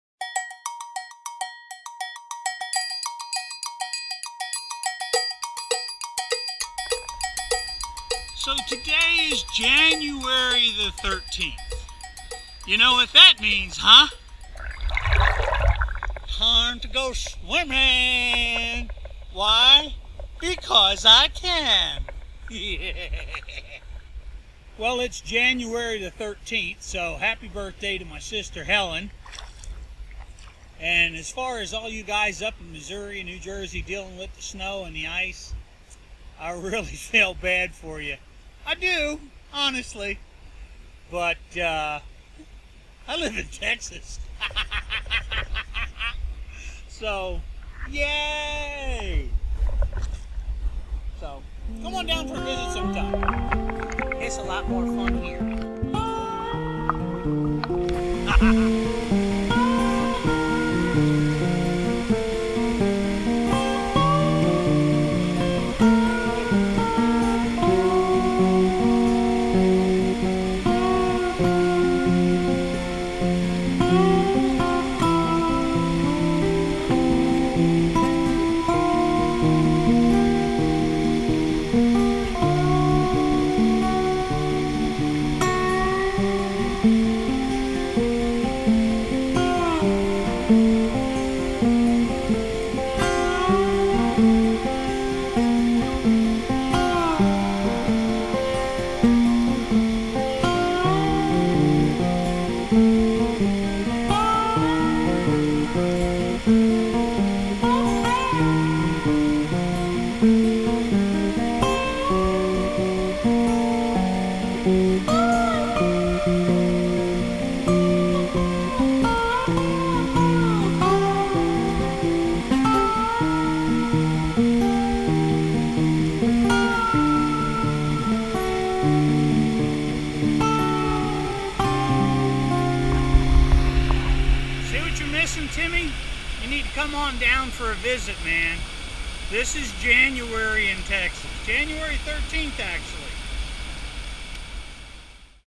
Jan 13 2017
It's Friday the 13th... January 13. My sister's birthday... and there is a big ice storm in Missouri, new Jersey, and parts of Canada... but I don't live there.  :- )